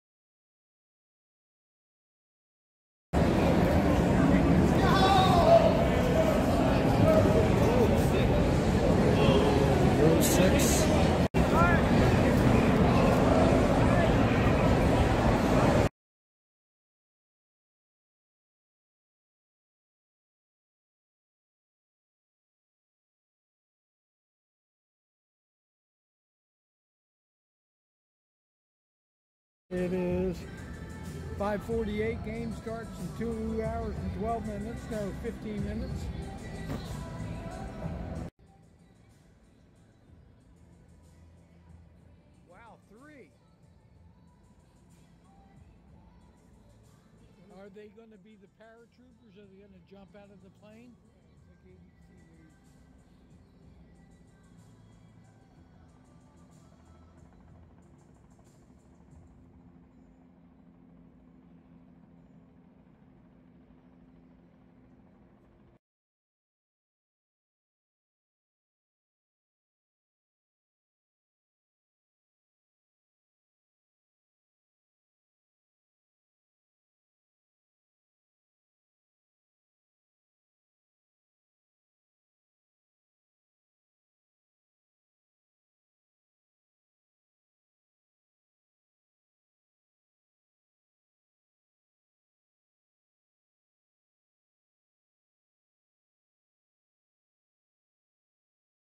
I was lucky enough to get a ticket for the World Series Game 3...An then this happened!
Not only did the game I was attending get rained out, This happened.